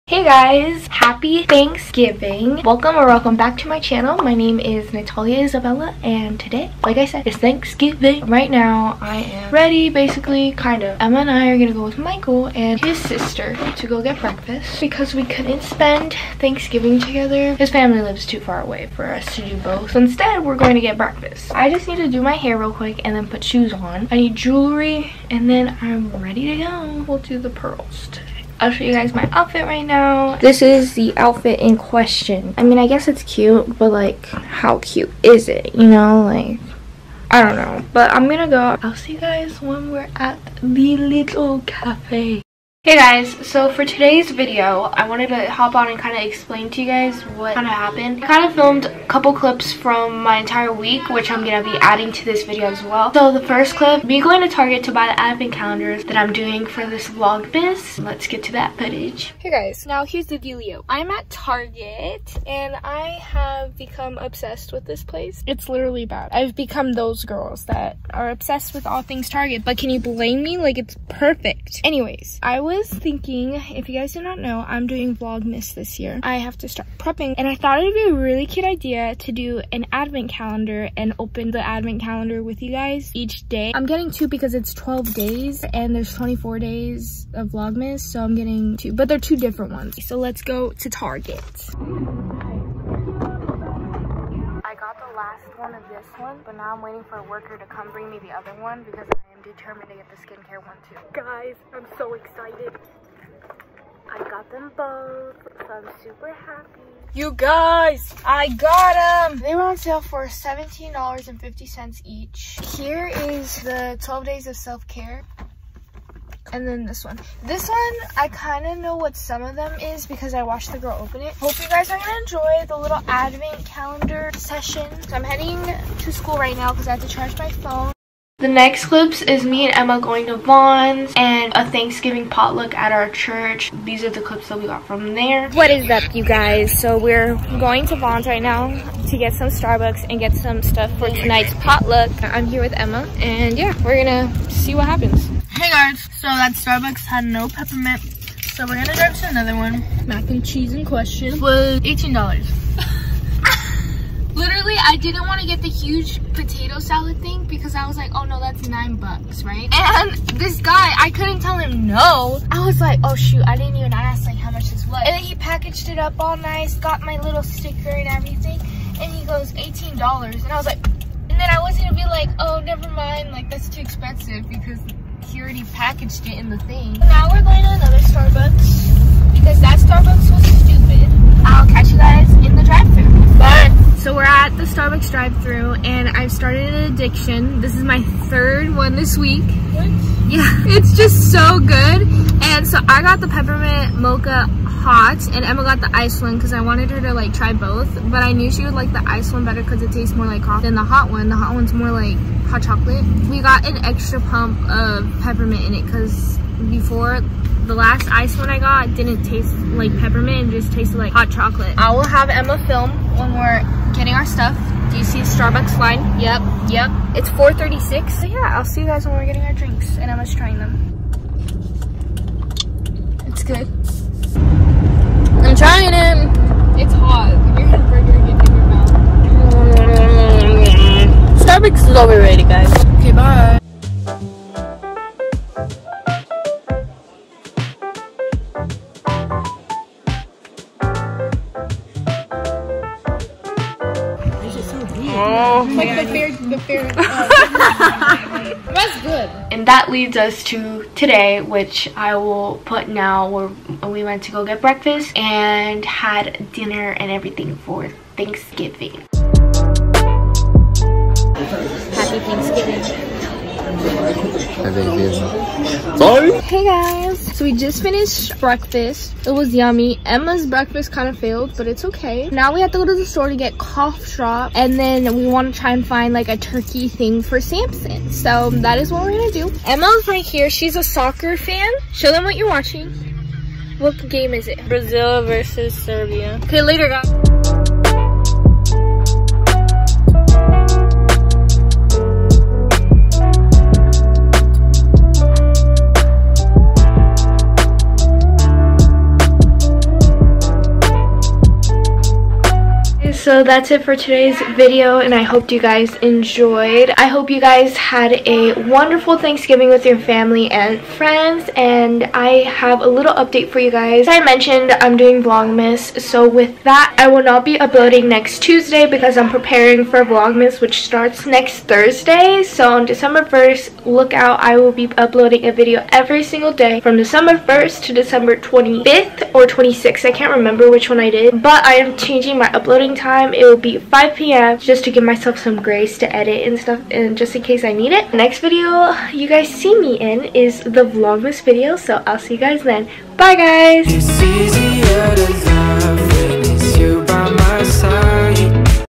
THANKSGIVING VLOG 2022!
Hi! In todays video I celebrated thanksgiving with my family and we ate so much food lol. I hope you guys had a wonderful Thanksgiving and I will see you guys next Thursday!

VLOGMAS SCHEDULE:
every day at 5 pm PST there will be a new video :)